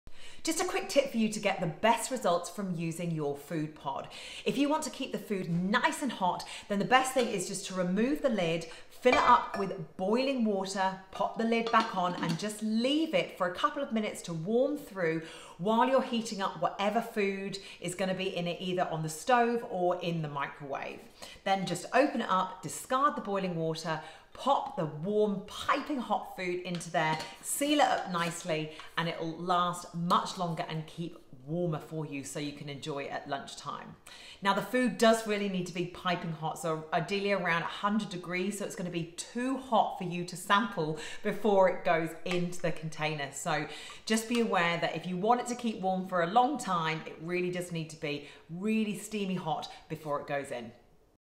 The new Oasis food pod
Watch my video to ensure you are getting the most out of your food pod and making sure your food is nice and hot at lunchtime!